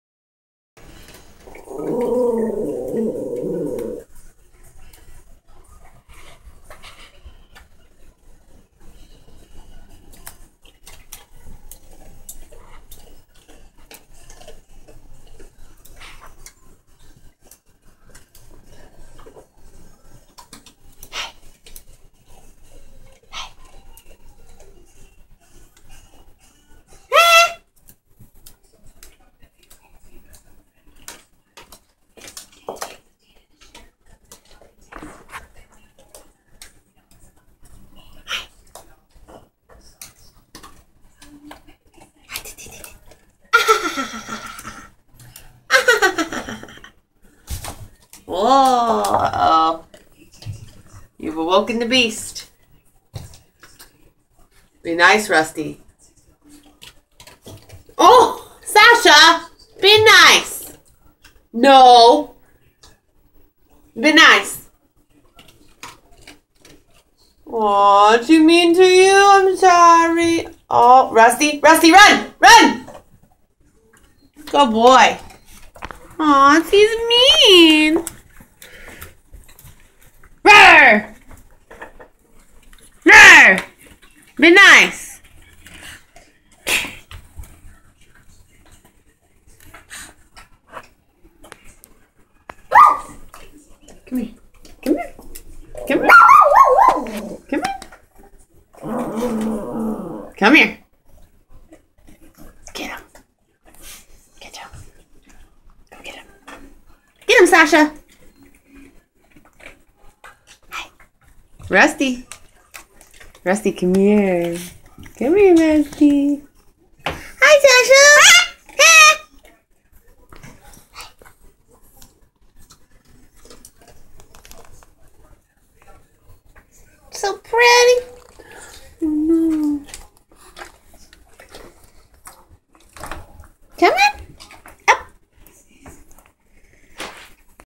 Poodle vs Cockatoo
Rusty and Sasha play fighting